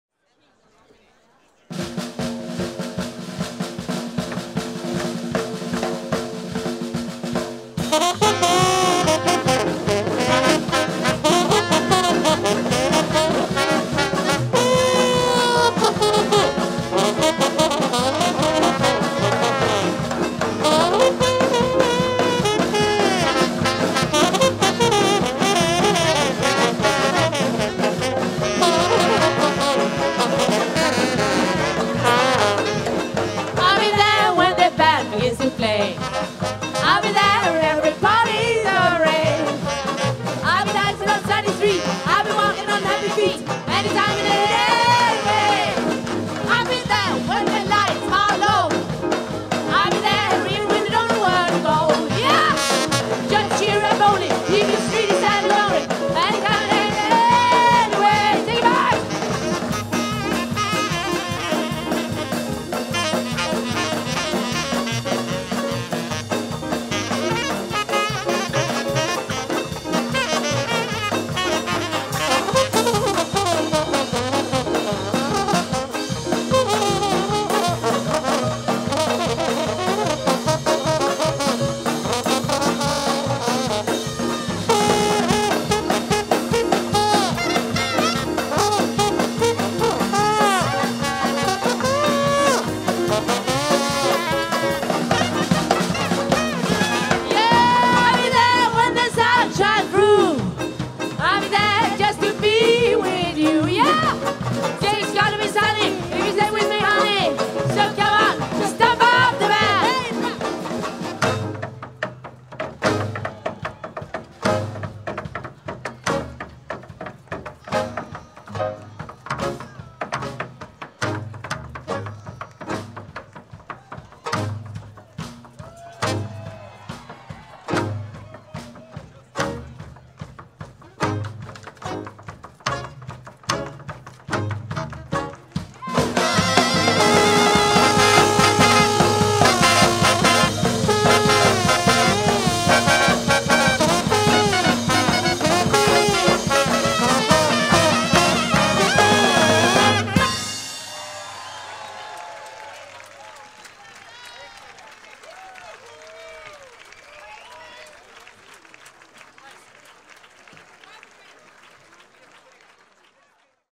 Policoro in Swing 2018 - Gunhild Carling & Carling Family
Concert of  Gunhild Carling & Carling Family  at Segni Ballroom
19/25 August 2018.
Policoro in Swing is a Festival where music, dance, sailing, sun and sea are "bouncing to the same tune".